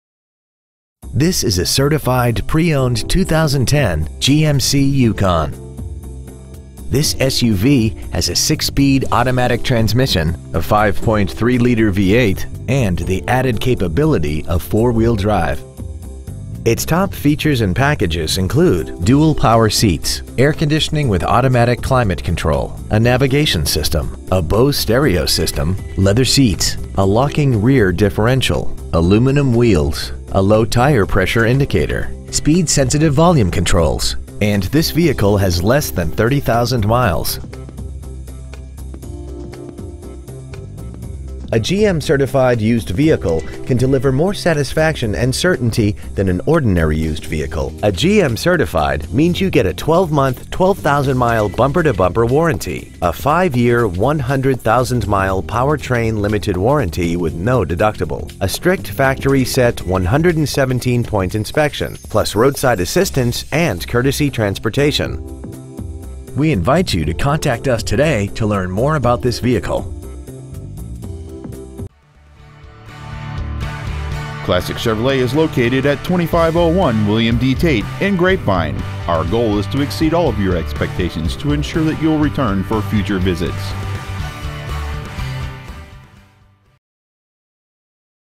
Certified 2010 GMC Yukon XL Ft. Worth TX
Year : 2010

 Make : GMC

 Model : Yukon XL

 Engine : Gas/Ethanol V8 5.3L/323

 Trans . : Automatic

 Exterior : Gray

 Miles : 29,241

 Interior : Tan

 Classic Chevrolet

 2501 William D Tate

 2010 GMC Yukon XL Grapevine TX